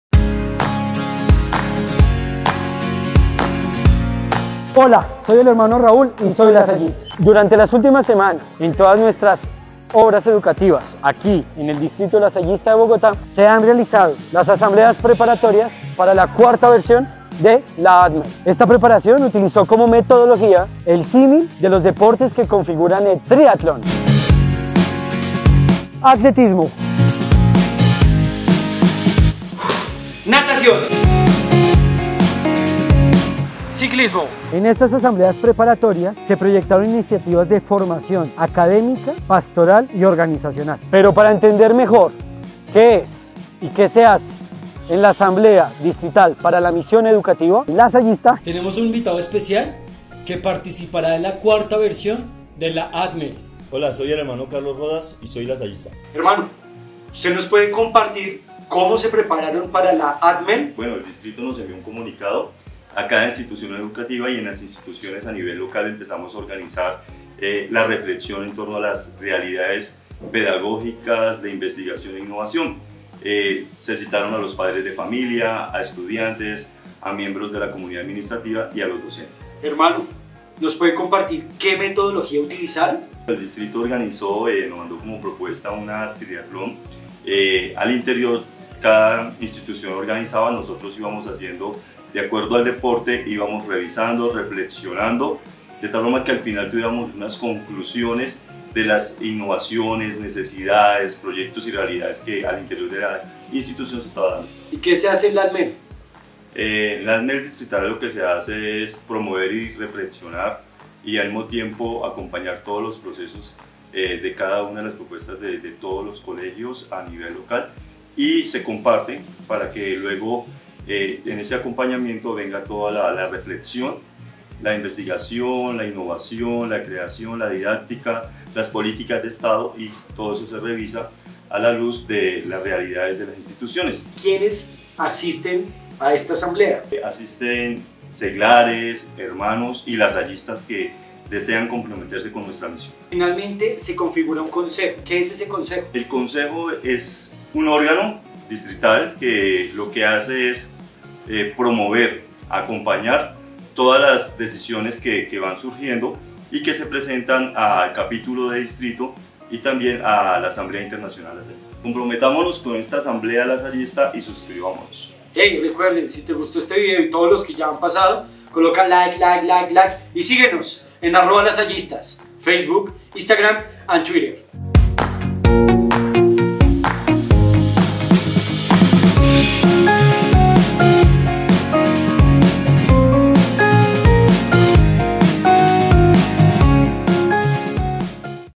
Tag ¿Qué es la de La ADMEL? Ft Hno Carlos Rodas
Durante las últimas semanas en todas nuestras obras educativas se realizó la Asamblea Preparatoria ADMEL en donde se proyectaron diferentes iniciativas de formación académica, pastoral y organizacional. El Hno. Carlos Rodas nos ayuda a entender mejor qué es y qué se hace en la Asamblea Distrital para la Misión Educativa Lasallista.

Producción Gráfica
Gina Jaramillo
Producción audiovisual
Andrés Felipe Tuta Hernández